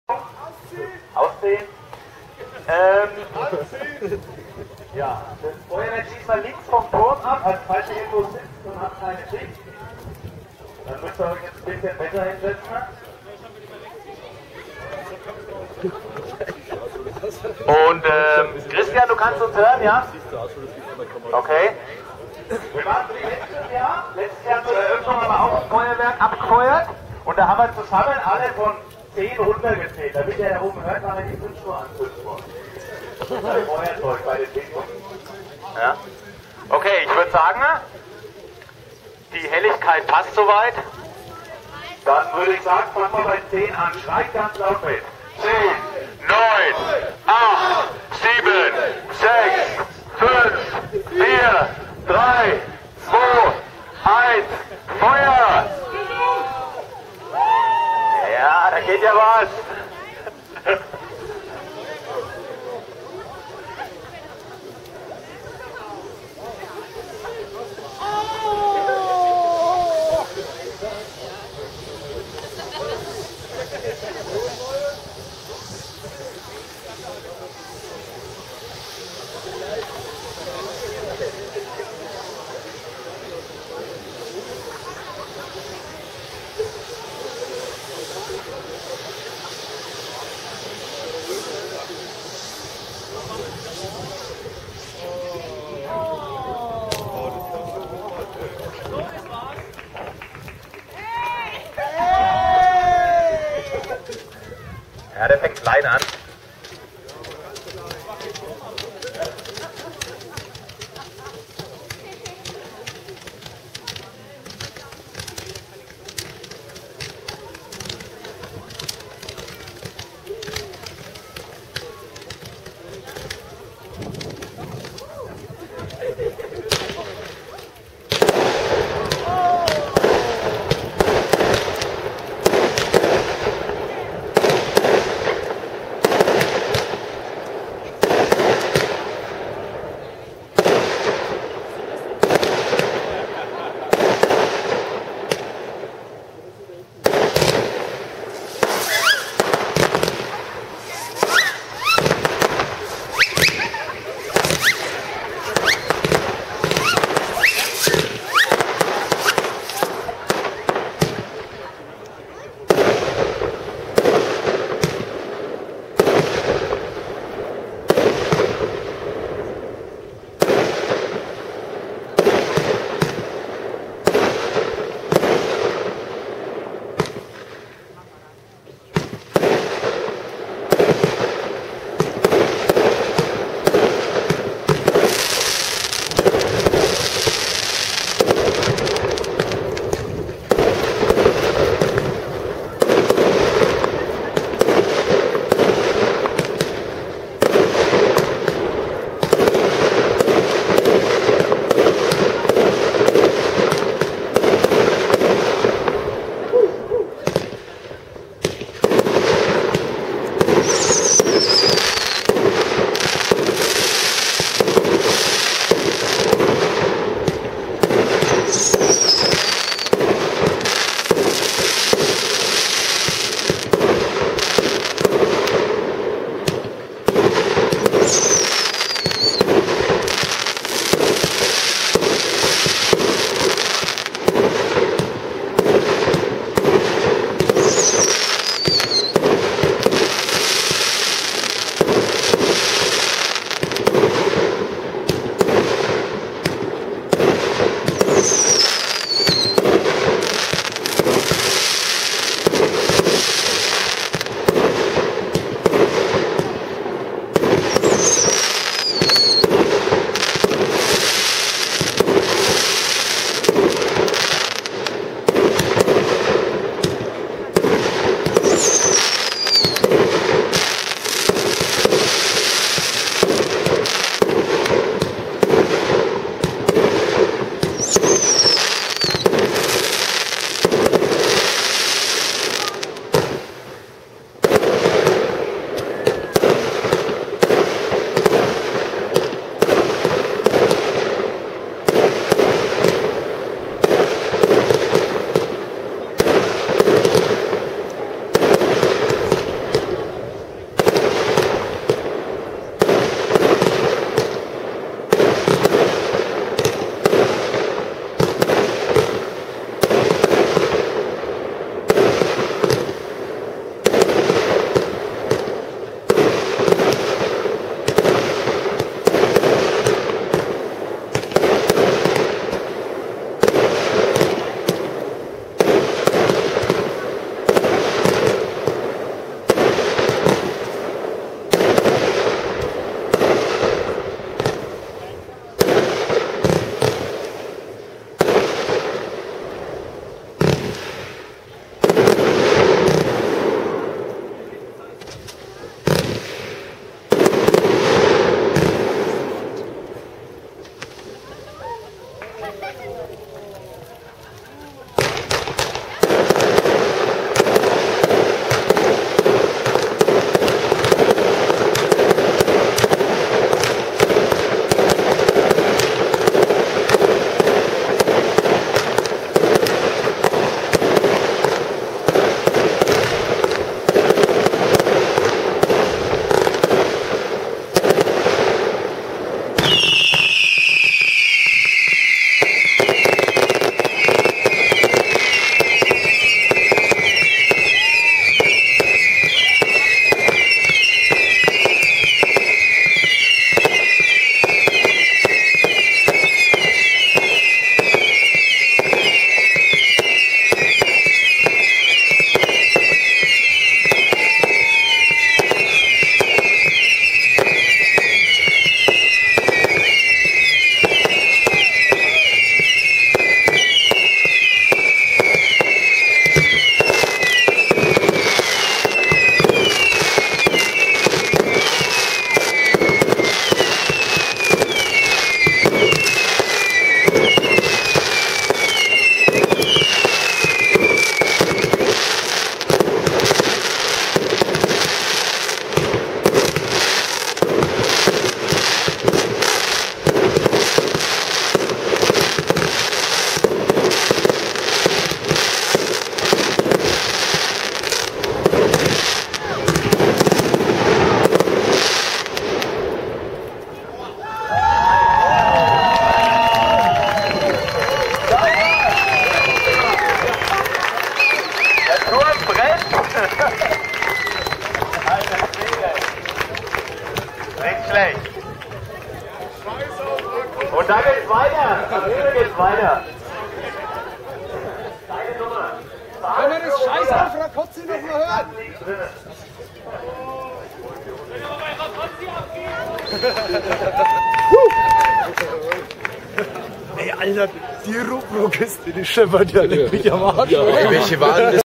Feuerwerk zum 1. Streetfoodfestival in Bad Kissingen - Saale Pyro-Müller
Dieses hübsche Ding hat mein Ex-Helfer hochgezogen und das hat er wirklich gut gemacht. Kissingen geht nun mehr und mehr in seine Hand über und das ist GUT so! Lokale Kräfte gehören unterstützt, wenn sie gut und engagiert sind und genau das ist hier der Fall. Alles Gute, Christian! Ich stehe dir zur Seite, wenn du mich brauchst :)